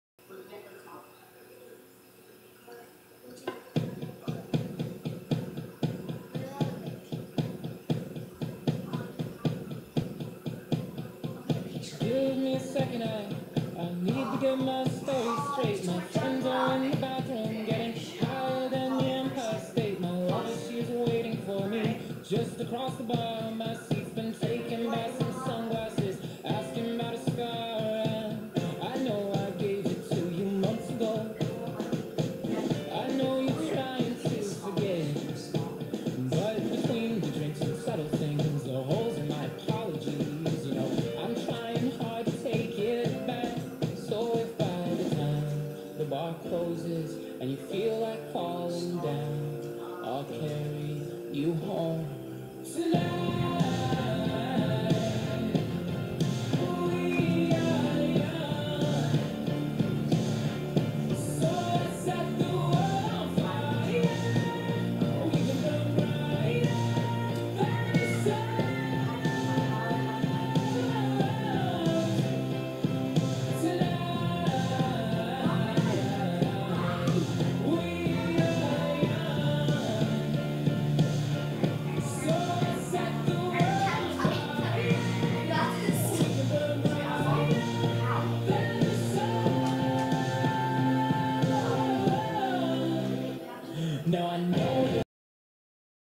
Photo Booth Randomness ♥
This is me and my awesomeee cousin skyler taking gangsta photos on photo booth :)